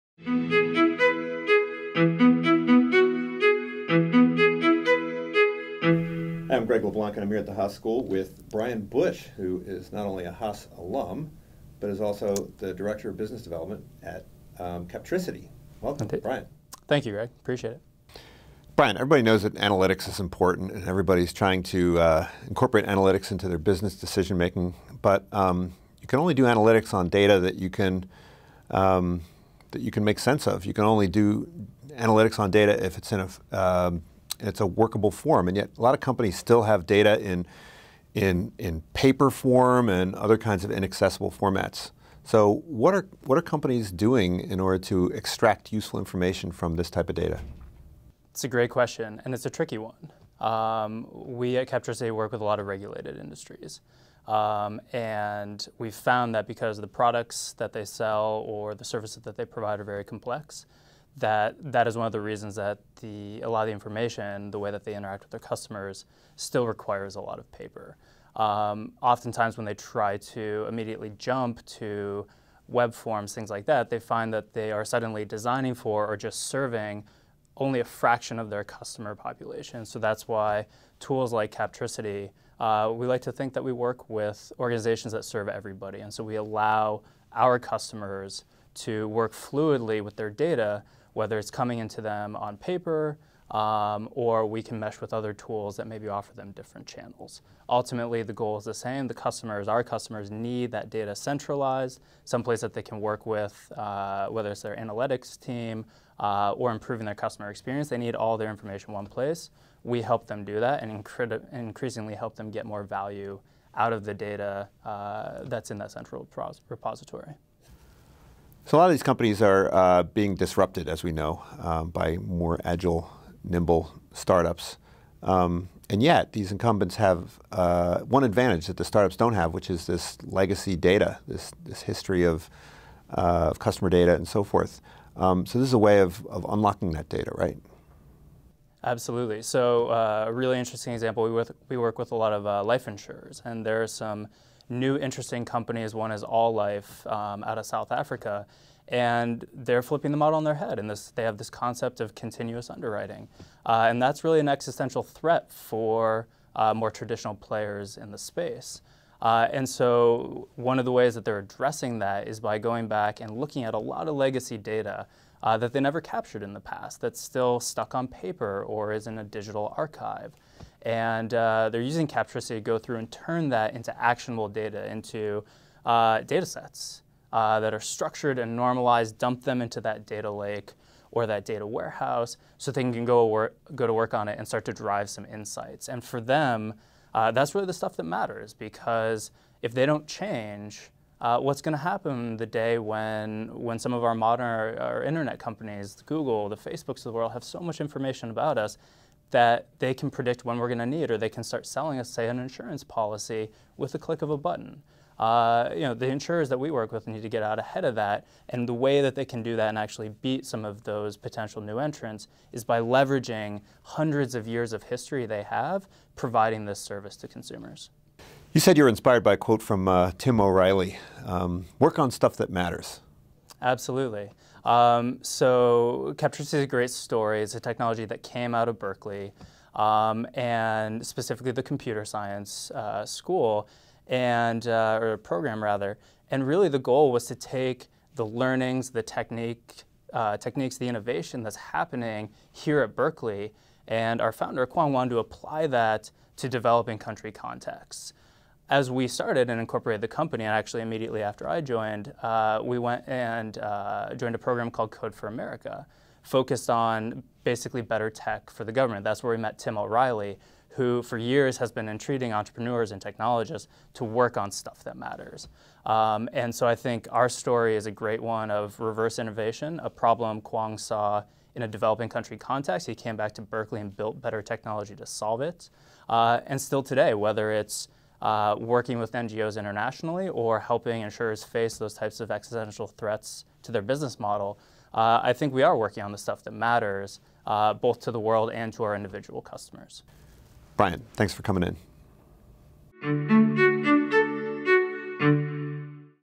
Brian Busch Captricity Interview 04/19/16 Data Science Speakers
Brian Busch is Director of Business Development at Captricity.

Held at the Haas School of Business, University of California, Berkeley, the Data Science & Strategy Lecture Series examines the evolving role of "big data" and analytics in managerial decision-making.  In this playlist, lecture series host Prof. Greg La Blanc interviews industry executives and practitioners on key topics in data science, including data mining, machine learning, visualization, advanced statistics and more.